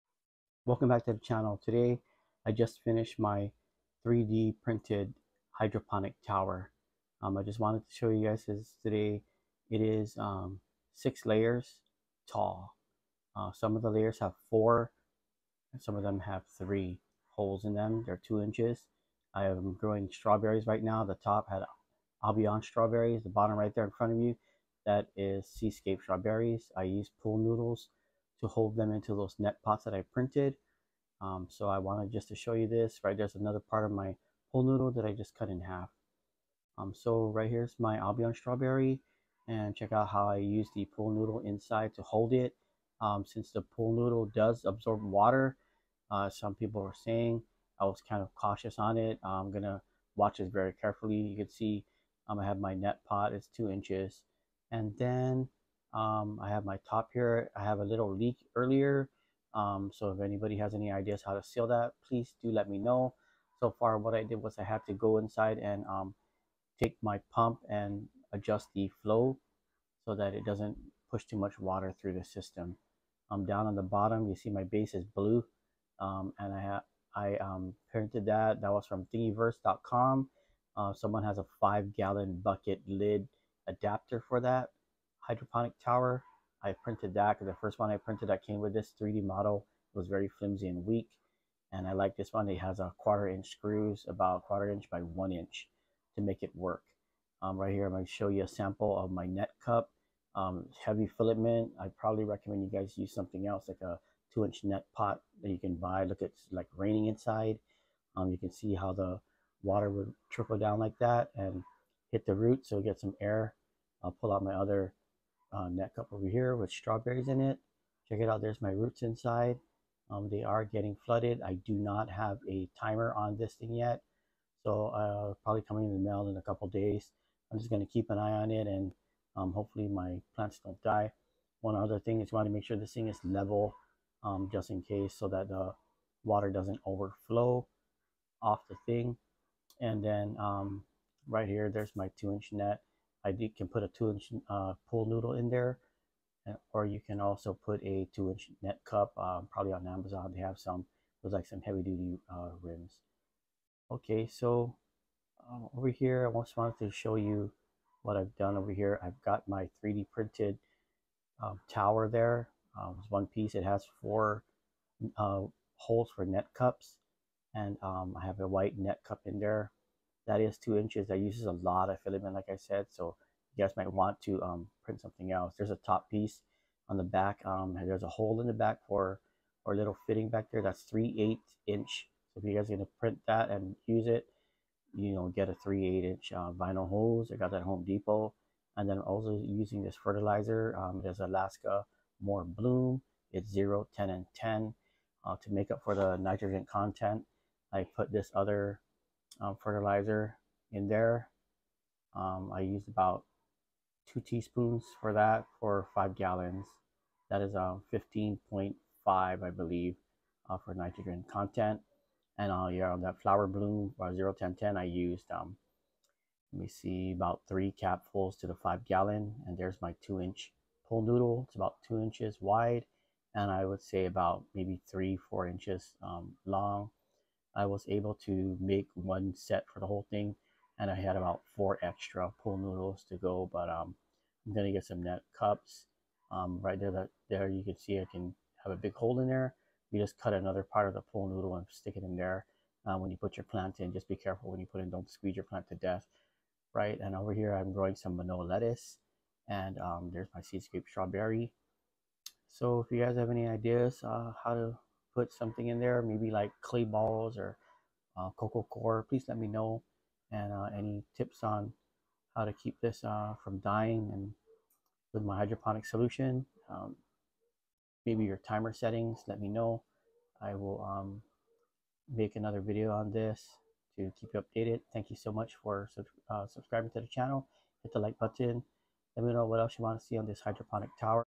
Grow Strawberries and Lettuce In 3D Printed Hydroponics Tower Even in The Winter Months
3D printed Hydroponic Grow tower with strawberries and lettuce.  Used Pool noodles to hold the plants to keep costs down. Want to grow during fall and winter months. Use a hydroponic tower!!